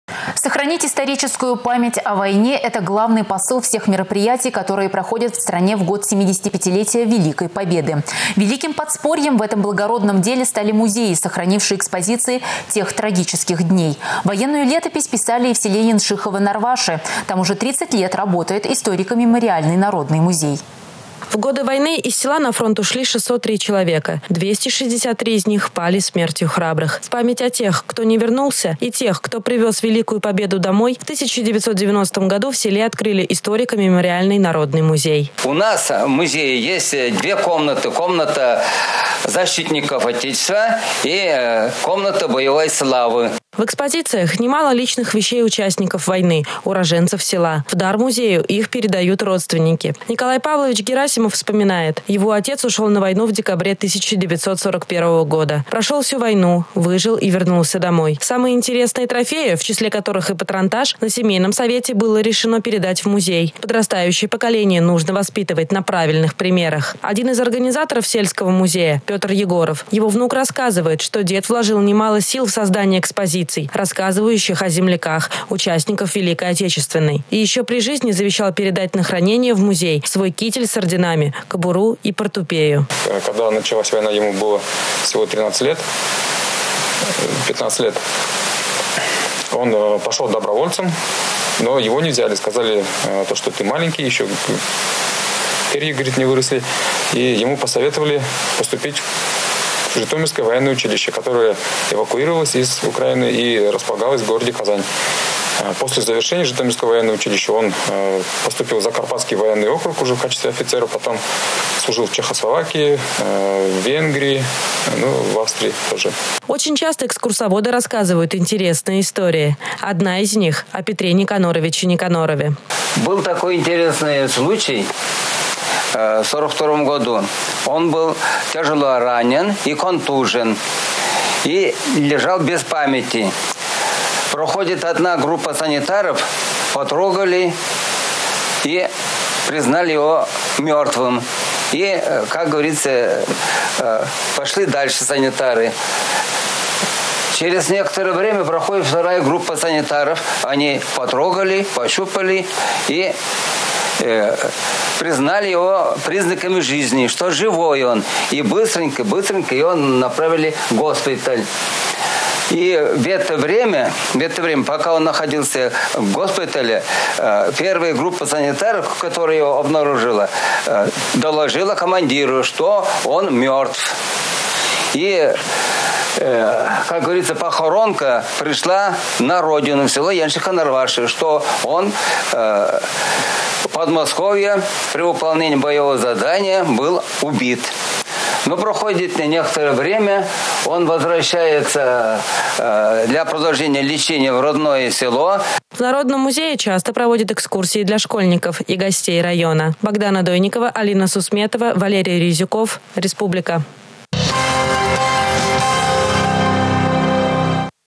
Музей в селе Яншихово-Норваши
Сохранить историческую память о войне -это главный посыл всех  мероприятий, которые проходят в стране в год 75-летия Великой Победы. Великим подспорьем в этом благородном деле стали музеи,  сохранившие экспозиции  тех трагических дней.   Военную летопись  писали и  в селе Яншихово-Норваши. Там   уже 30 лет работает историко-мемориальный народный музей.